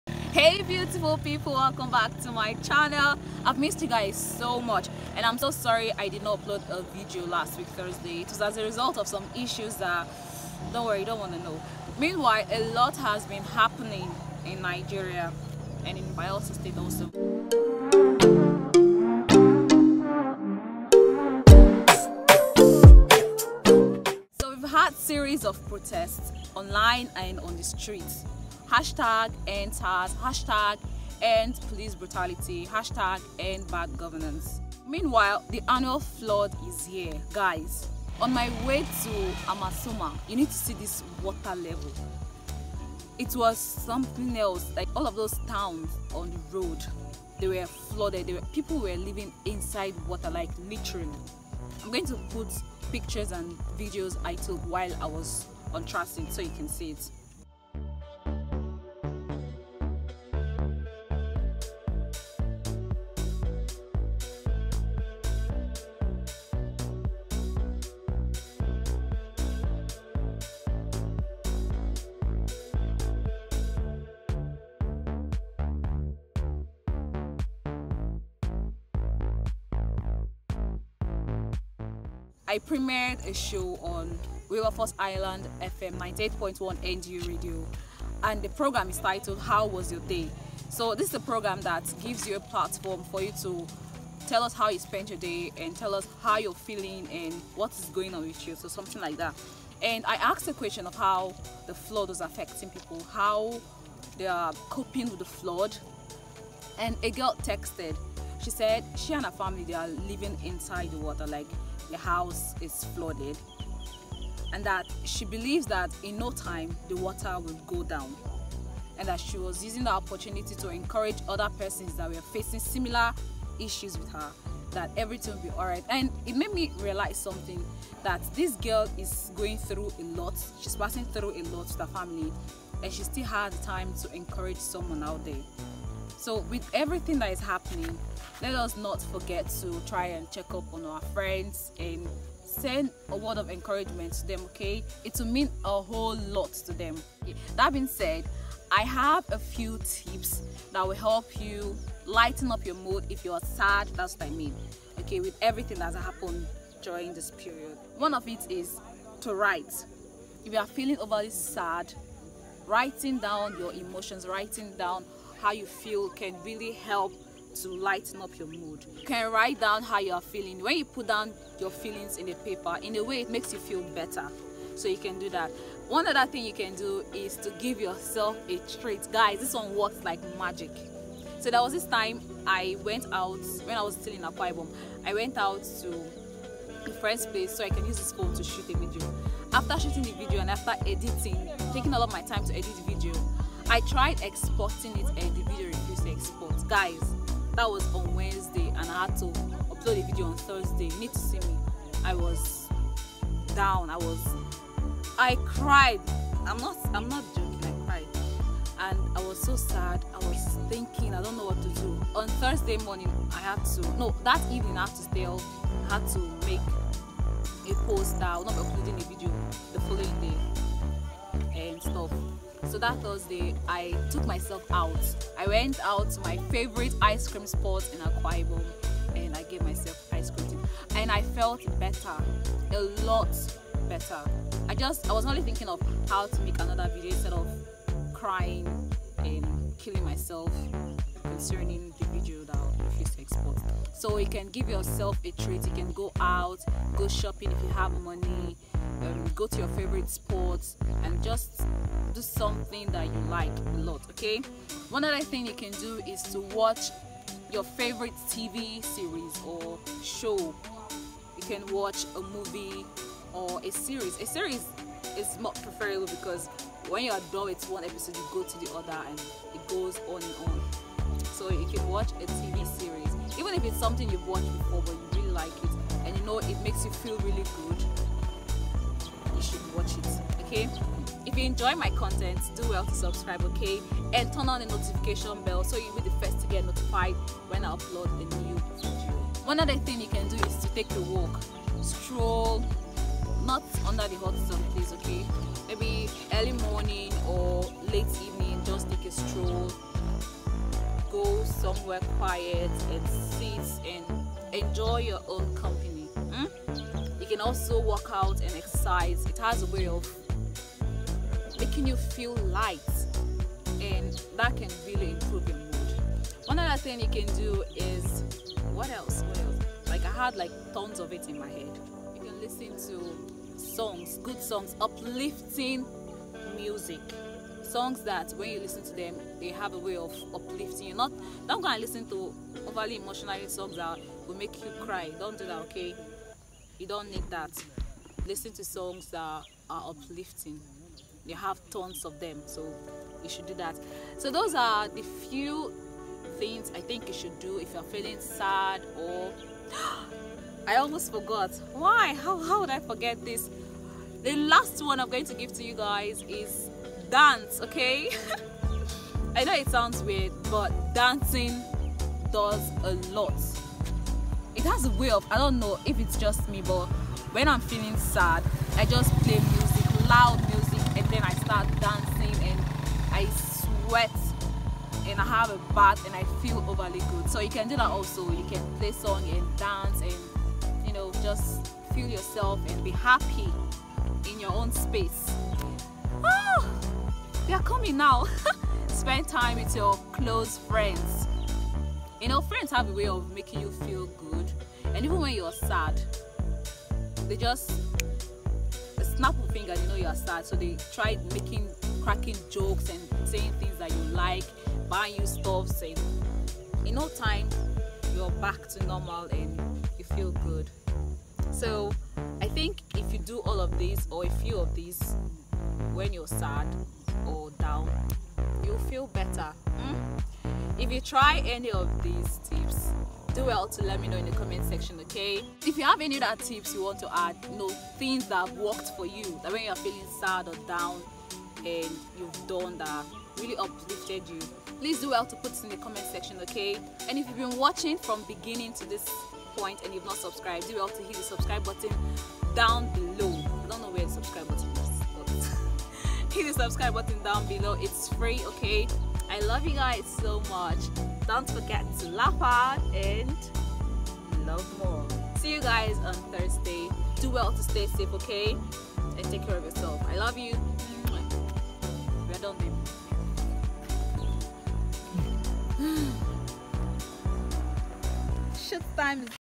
THINGS YOU SHOULD DO WHEN YOU FEEL SAD (End Police brutality/Flood)
In the past few weeks, We( Nigerians) have experienced a lot. Following the increase rate of police brutality in the country, there was need to say, no to police harassment. There was a peaceful protest in early October (online and offline) and towards the end of October, over 60 peaceful protesters in the country died while protesting against endsars endpolicebrutality endbadgovernance 🇳🇬
This made a huge number of us go into a dark place. With all of these still happening, we were visited by the annual flood, which has also taken the life of a few people.
I decided to make a video talking about THINGS THAT WE CAN DO TO LIGHTEN OUR MOOD WHEN WE ARE FEELING DOWN (SAD). This video is dedicated to all those who are feeling down (sad) in one way or the other.

IF YOU HAVE ANY OTHER TIPS, do well to share in the comment section, it might be of HELP to someone.
Songs

Musician: Jef
Source: InShot